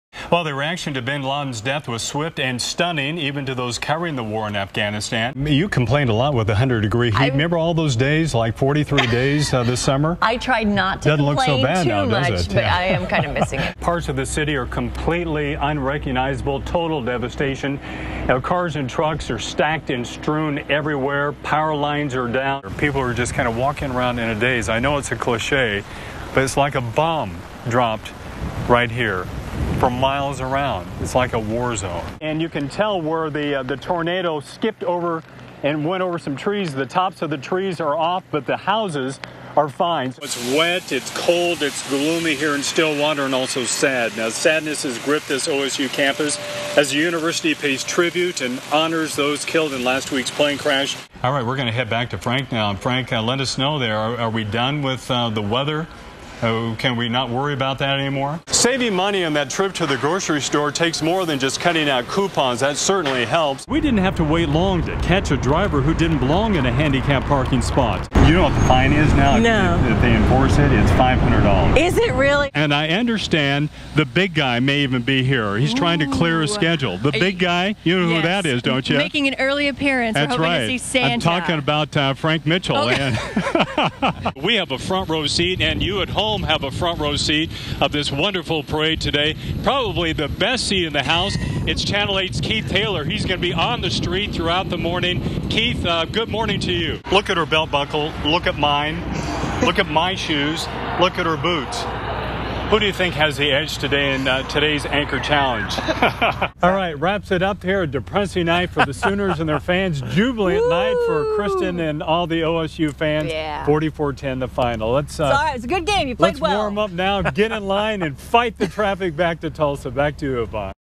Mark Bradshaw Montage 1-12
Mark Bradshaw is a primary news anchor for KTUL-TV in Tulsa, OK.